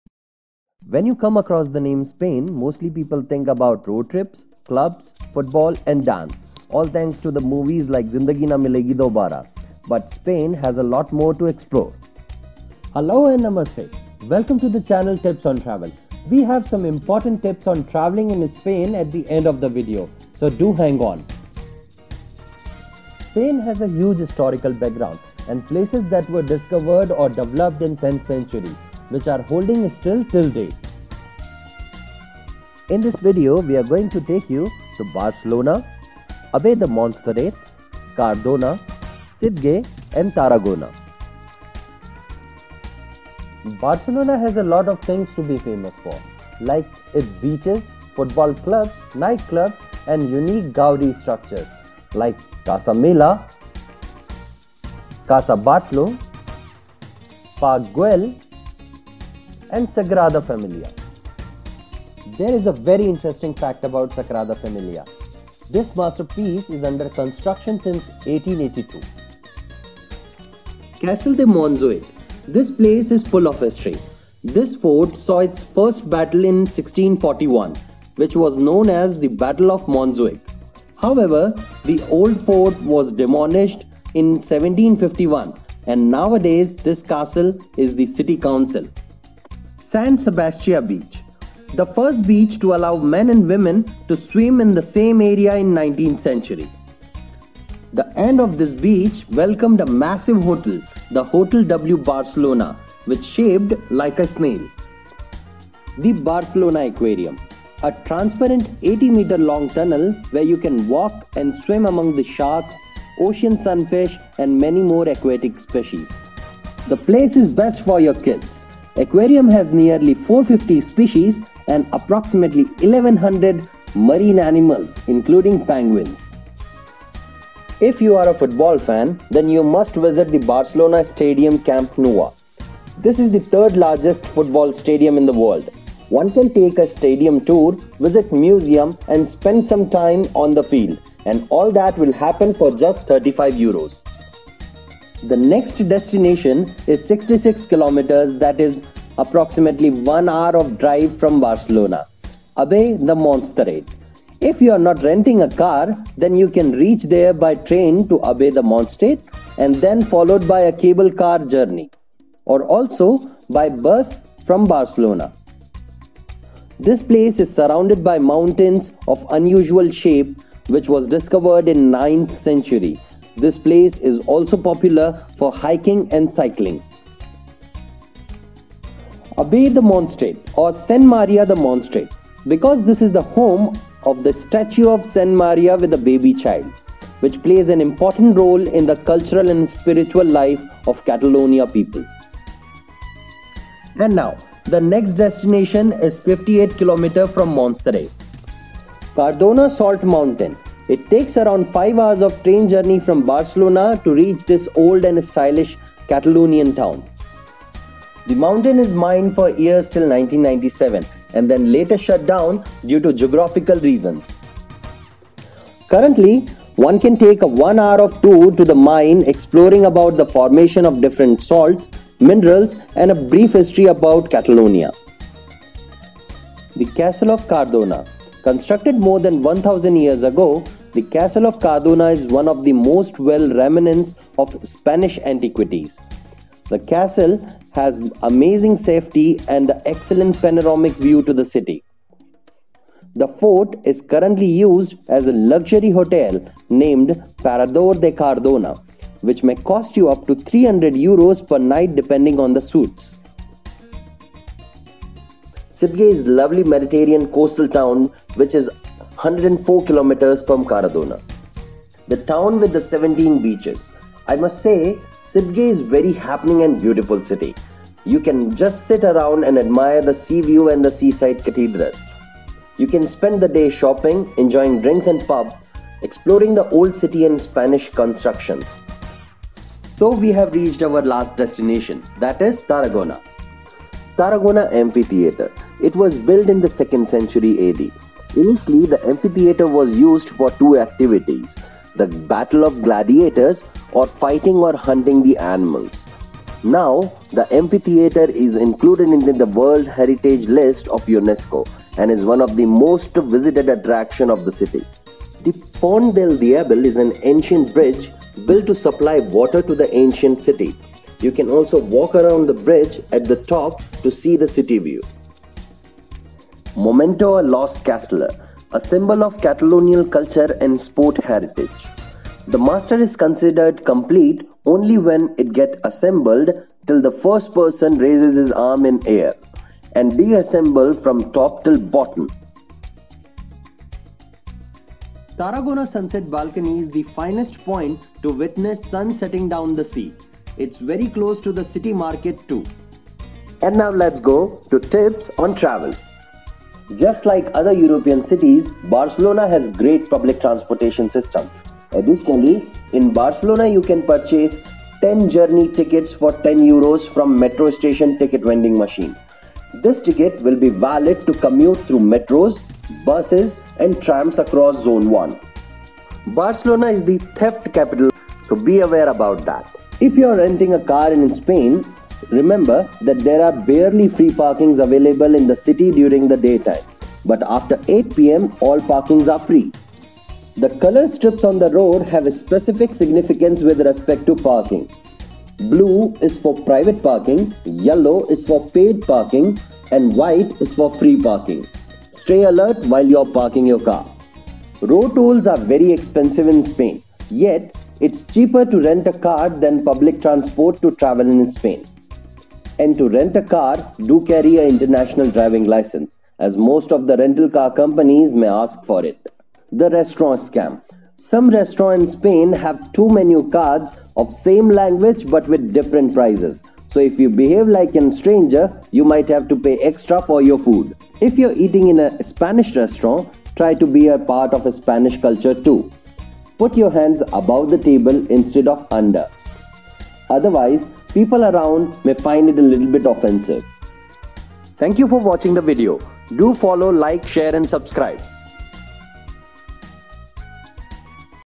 Spain | Hidden Gems | Travel in Spain | Road Trips | Places to visit Spain | GhoomBees
Spain is famous for its road trips and places with magistic beauty. This video is about some of the hidden gems in spain which are likely less famous but have a lot to explore.